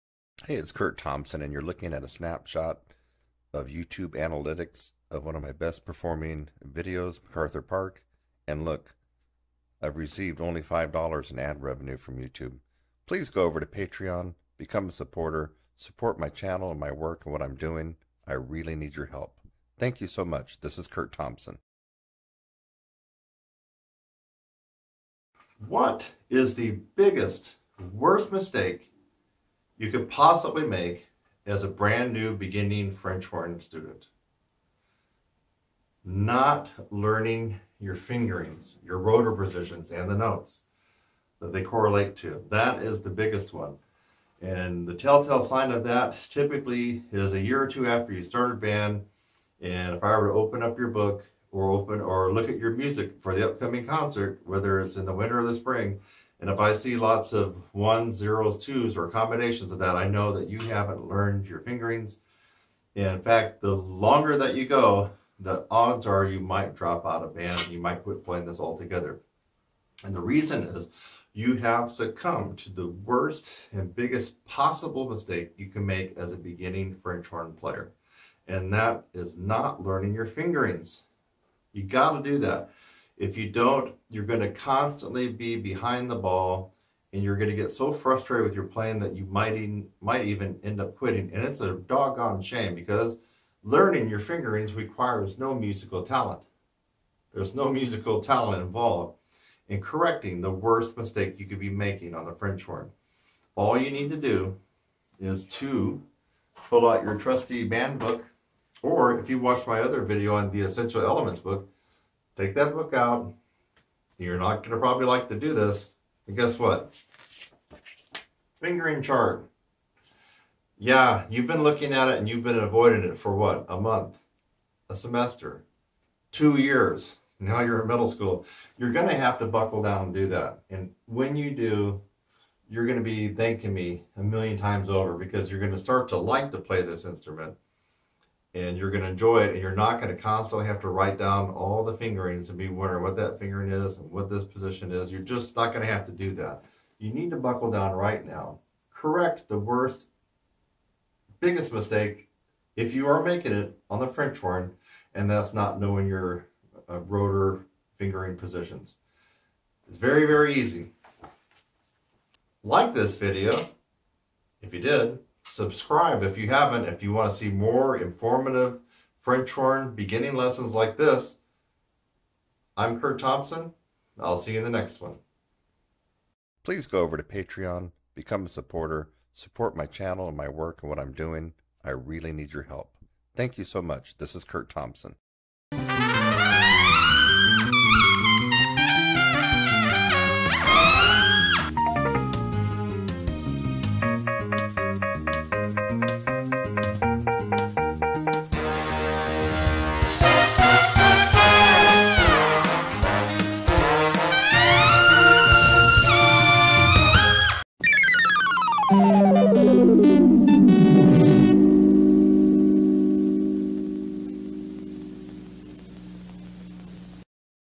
Worst Mistake a New French Horn Student Can Make! Free French Horn Lessons
Kurt Recommends the Mendini Single French Horn for beginners that you saw him play in this video and the best beginner book Kurt recommends for French Horn is Essential Elements French Horn Book 1